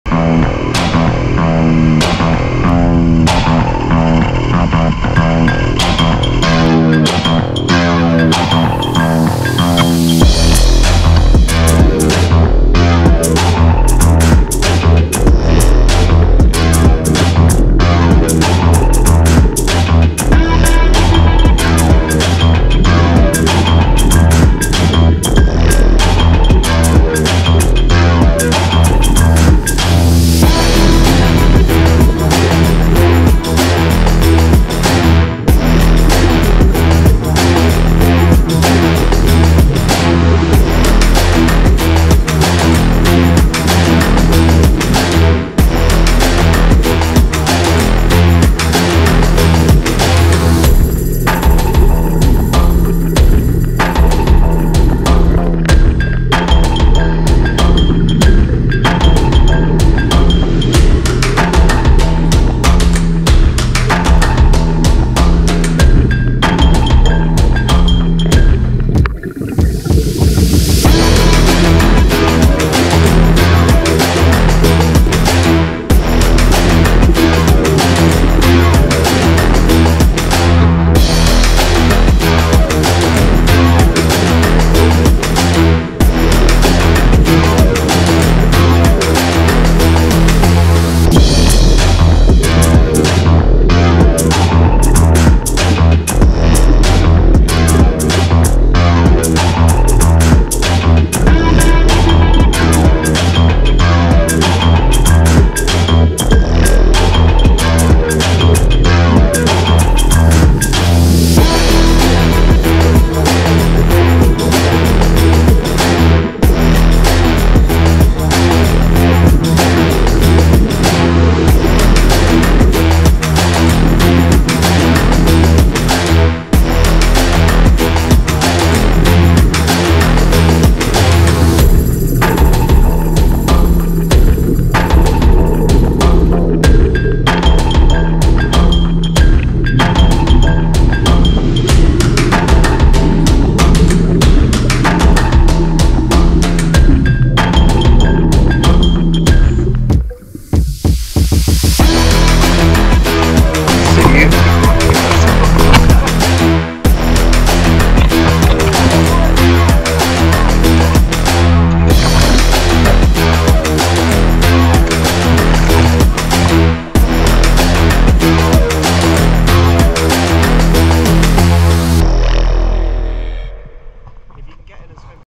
Exploring a SUNKEN FERRY | The Countess of Erne
The fourth and final boat dive at Portland, we dived the Countess of Erne wreck, situated at the North East breakwater of Portland Harbour.

Dive Location - Portland Harbour, Devon (UK)

Featured Links:
Music Credit - Music from Uppbeat (free for Creators!):

Rig:
Video - GoPro Hero 9 Black, Waterproof Housing, Yellow "Floaty"

SCUBA - Mares Open Water Regulators, Mares 7mm Wetsuit, Aqua Lung i100 Dive Computer